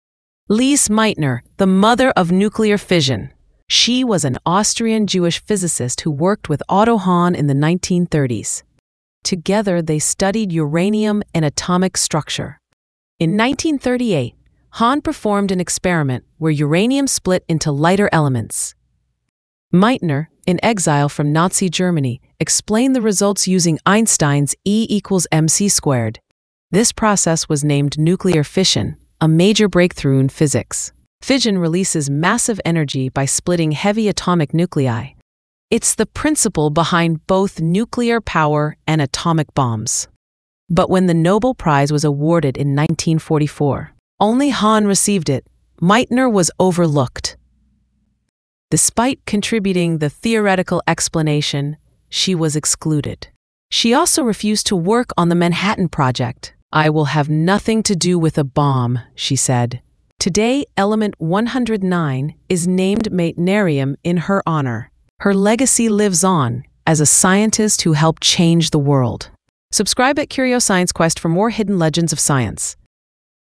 Discovery of Nuclear Fission: Lise Meitner||Co-Discoverer of Nuclear Fission
The Hidden Legacy of Lise Meitner | Element 109 is named Meitnerium in her honour  but few know why Lise Meitner refused to work on it. Her story is one of brilliance, integrity, and scientific revolution.